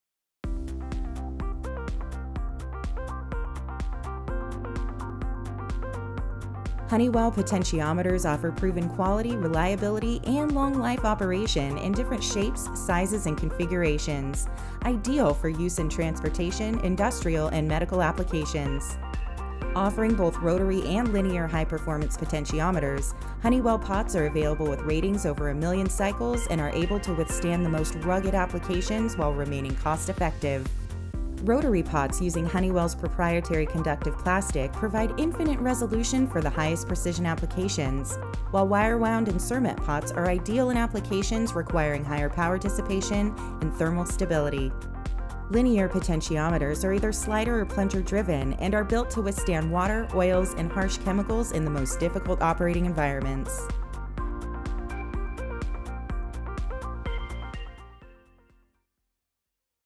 Component Moment - Honeywell Potentiometers
Honeywell potentiometers offer proven quality, reliability and long life operation in different shapes sizes and configurations. Ideal for use in transportation, industrial and medical applications. Offering both rotary and linear high performance potentiometers, Honeywell pots are available with
ratings over a million cycles and are able to expand the most rugged applications while remaining
cost-effective. Rotary pots using Honeywell's proprietary conductive plastic provide infinite  resolution for the highest precision application; while wire-wound and thermos pots are ideal in
applications requiring higher power dissipation and thermal stability. Linear potentiometer or either slider or plunger driven and are built to withstand water. oils and harsh chemicals in the most difficult operating environment.

Honeywell Potentiometers

All Honeywell potentiometers are built for rugged, long-lasting rotational performance – in some cases more than a million cycles. Potentiometers are available in cermet, wire wound, metal, plastic, or combination packages, sealed against dust or moisture, and sized to fit into tight spaces. A variety of terminations and flexible mounting options are available to meet your needs.